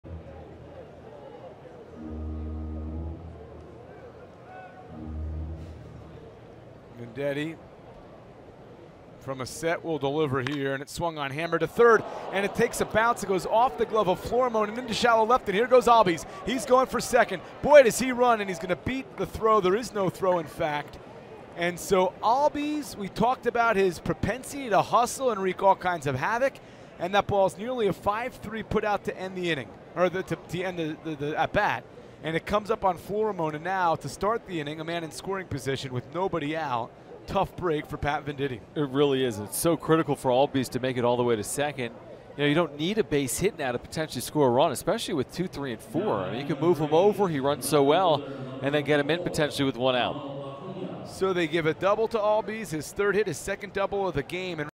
Gwinnett's Albies hits second double
7/27/17: Gwinnett shortstop Ozzie Albies starts off the free innings by ripping his second two-bagger of the contest